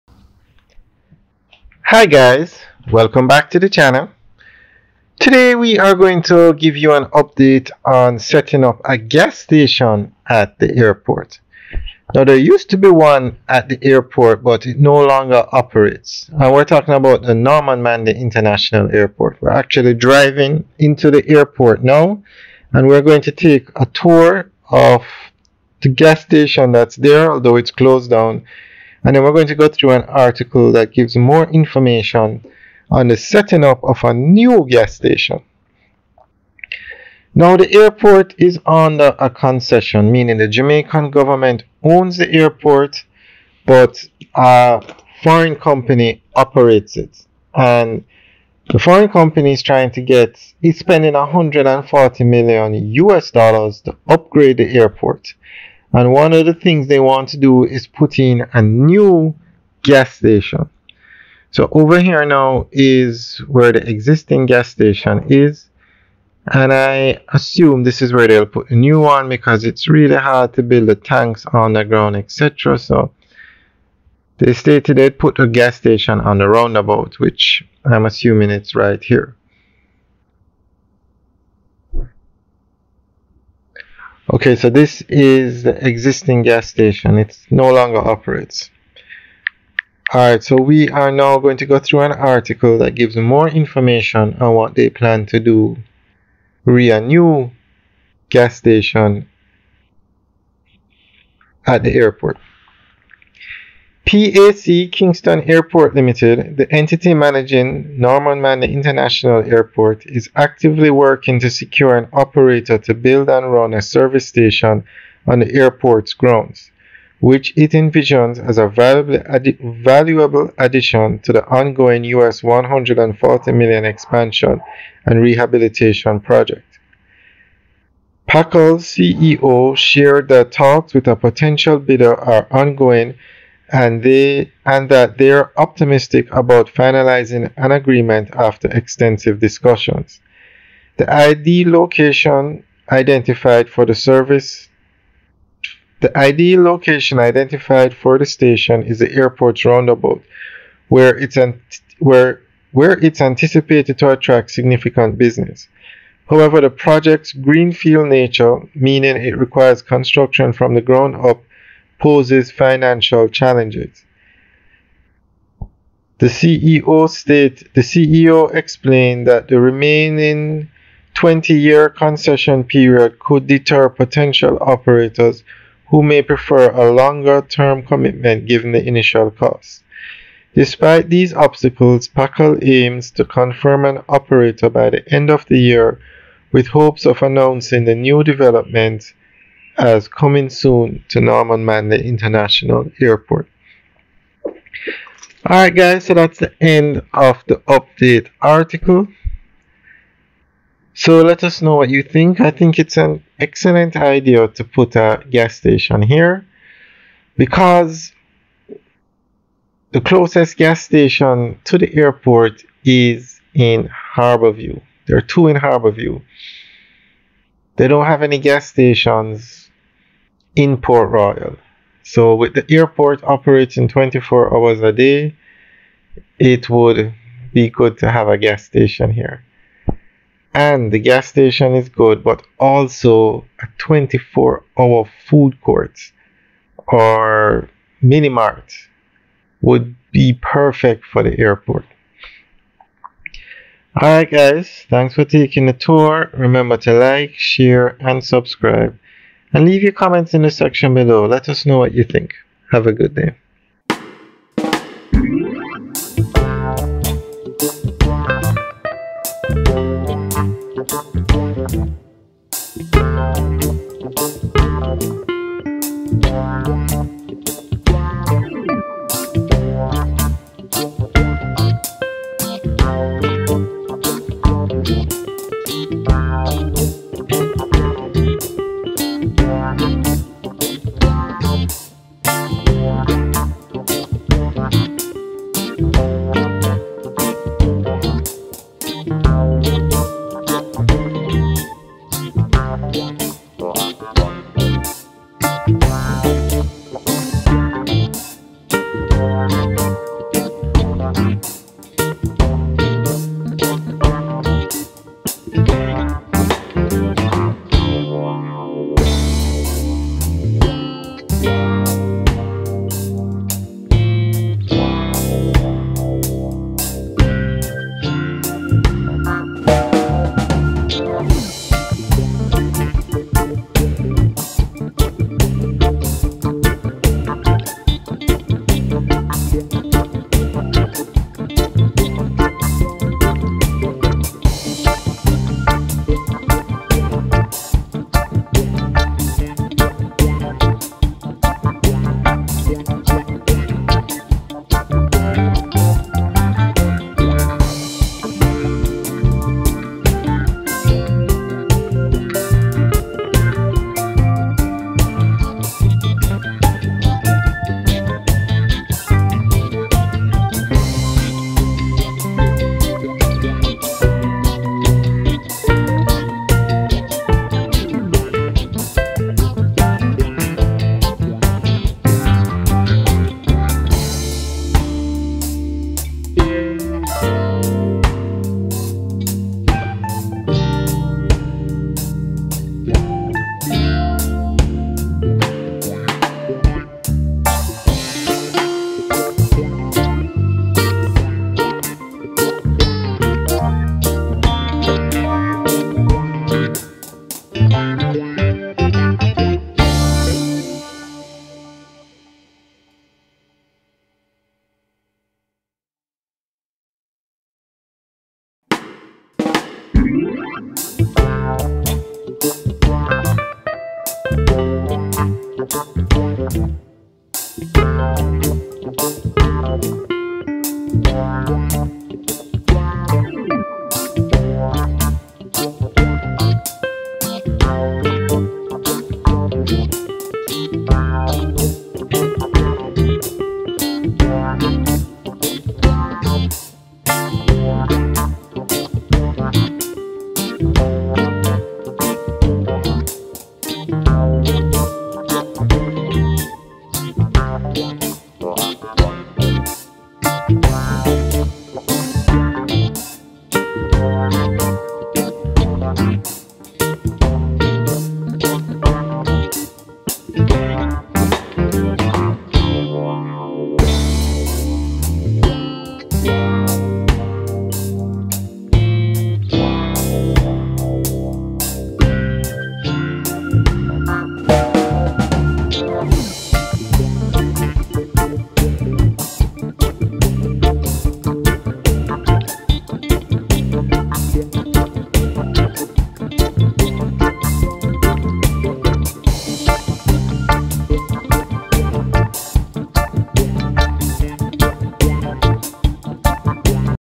Construction New Gas Station NMIA Jamaica
A narrated driving tour Construction New Gas Station Norman Manley International Airport Jamaica